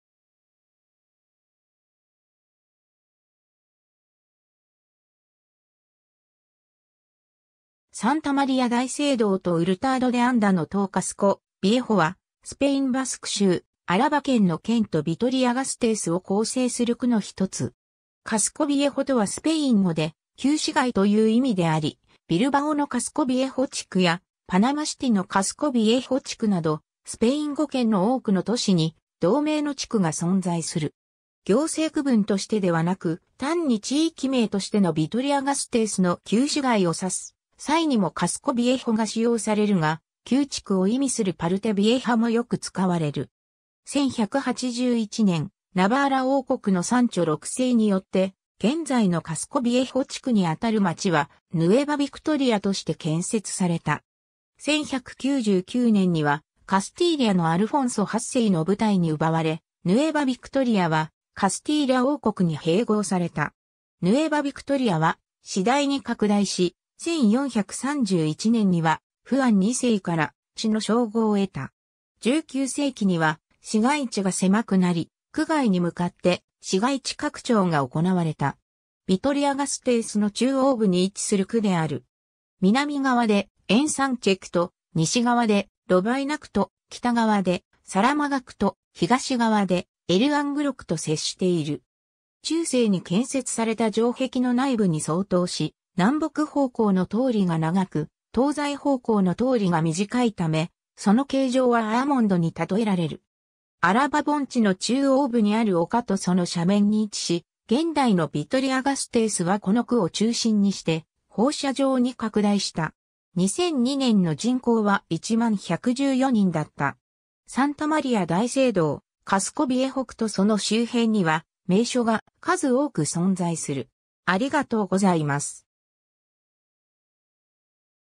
カスコ・ビエホ (ビトリア＝ガステイス)
サンタ・マリア大聖堂とウルタード・デ・アンダの塔 カスコ・ビエホ(スペイン語: Casco Viejo)は、スペイン・バスク州アラバ県の県都ビトリア＝ガステイスを構成する区のひとつ。
カスコ・ビエホとはスペイン語で「旧市街」という意味であり、ビルバオのカスコ・ビエホ地区やパナマシティのカスコ・ビエホ地区など、スペイン語圏の多くの都市に同名の地区が存在する。
行政区分としてではなく単に地域名としてのビトリア＝ガステイスの旧市街を指す際にもカスコ・ビエホが使用されるが、「旧地区」を意味するパルテ・ビエハ(スペイン語: Parte Vieja)もよく使われる。
 1181年、ナバーラ王国のサンチョ6世（賢王）によって、現在のカスコ・ビエホ地区にあたる町はヌエバ・ビクトリア(Nueva Victoria)として建設された。
1199年にはカスティーリャのアルフォンソ8世の部隊に奪われ、ヌエバ・ビクトリアはカスティーリャ王国に併合された。
ヌエバ・ビクトリアは次第に拡大し、1431年にはフアン2世から「市」の称号を得た。
19世紀には市街地が狭くなり、区外に向かって市街地拡張が行われた。
 ビトリア＝ガステイスの中央部に位置する区である。
南側でエンサンチェ区と、西側でロバイナ区と、北側でサラマガ区と、東側でエル・アングロ区と接している。
中世に建設された城壁の内部に相当し、南北方向の通りが長く、東西方向の通りが短いため、その形状はアーモンドに例えられる。
アラバ盆地の中央部にある丘とその斜面に位置し、現代のビトリア＝ガステイスはこの区を中心にして放射状に拡大した。
2002年の人口は10,114人だった。
 サンタ・マリア大聖堂 カスコ・ビエホ区とその周辺には名所が数多く存在する。